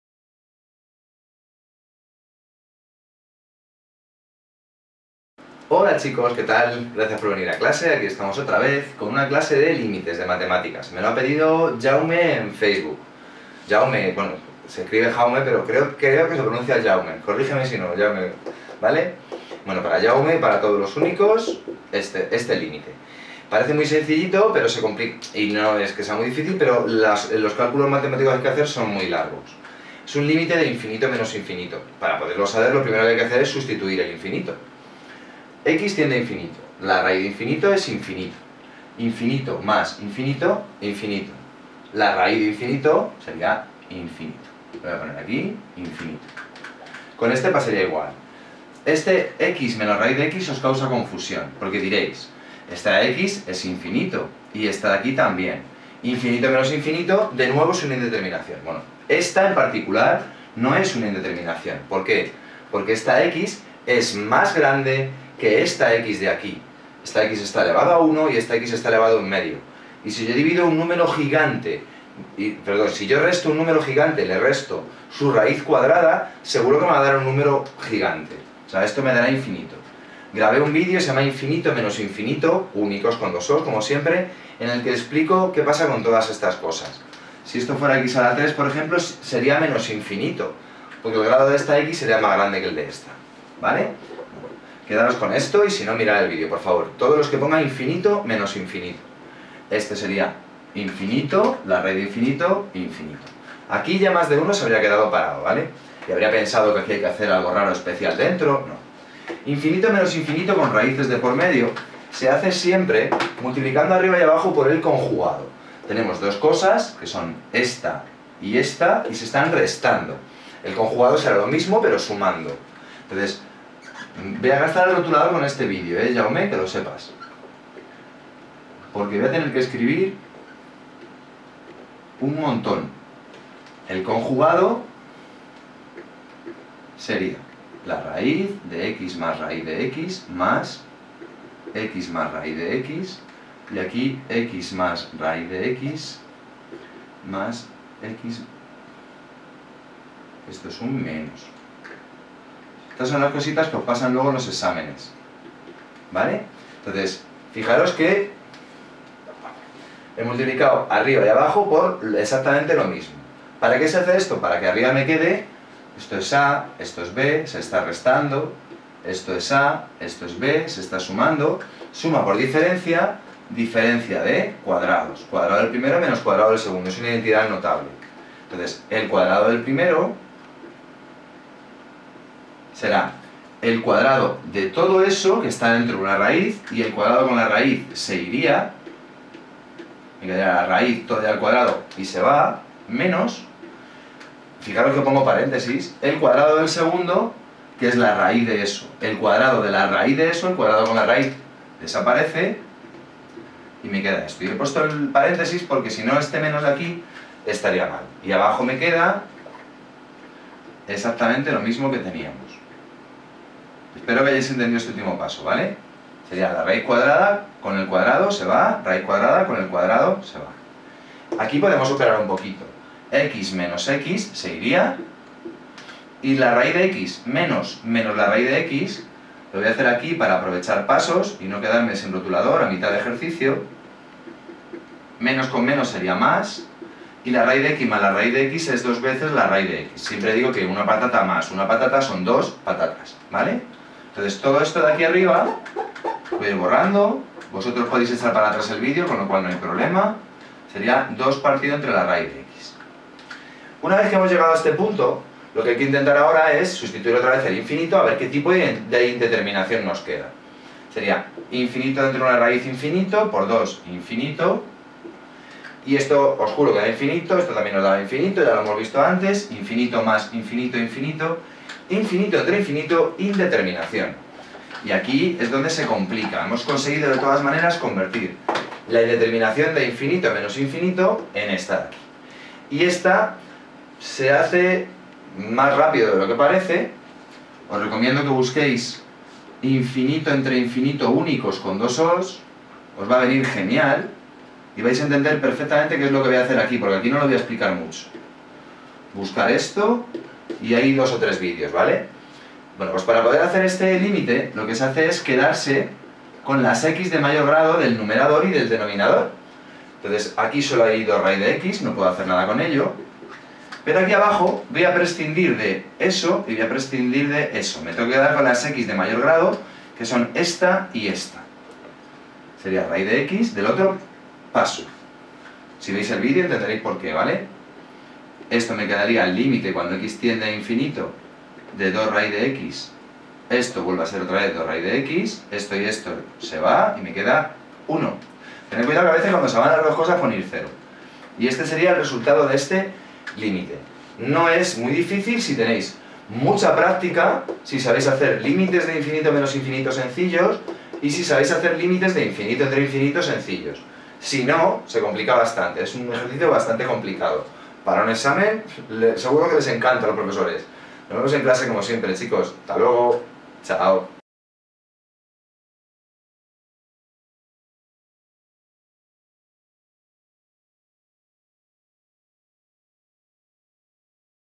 Limite infinito menos infinito 02 BACHILLERATO matematicas
Correspondiente a 1º de BACHILLERATO, resolveremos algunos limites de la indeterminación infinito menos infinito.

Puedes echar un vistazo a otros videos similares en esta sección de la web.

Y si quieres ayudarme a subtitular los vídeos a otros idiomas, te lo agradeceré infinito...